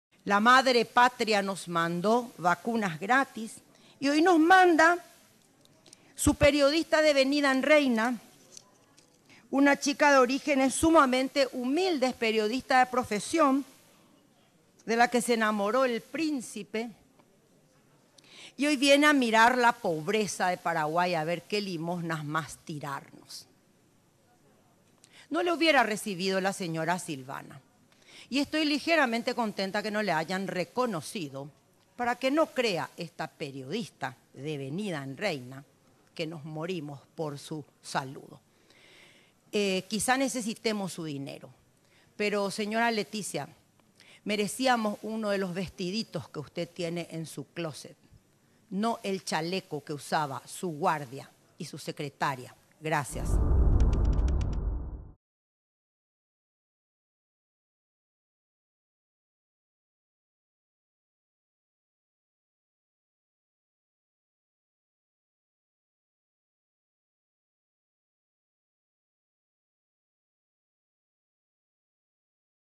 Ataca a Letizia por llevar puesto un chaleco de cooperante y no "uno de sus vestiditos"
Los modelitos de la Reina Letizia siempre dan que hablar. La Reina, con un chaleco de cooperante de la Cruz Roja visitaba Paraguay y esta vez, su vestimenta ha llegado al parlamento del país.
La diputada paraguaya Celeste Amarilla, del Partido Liberal Radical, lo consideró un desprecio a su país. "Vino con ropa ni siquiera de calle, de uniforme... me pregunto hasta dónde esto en la jerga protocolar no es un desprecio. Por un lado alabo la sencillez de la señora, pero por otro me molesta que cuando va a otros países lleva atuendos que fácilmente superan los diez mil dólares, haciendo así honor al lugar que visita. Señora Letizia, merecíamos uno de los vestiditos que usted tiene en su closet, no el chaleco que usaba su guardia o su secretaria".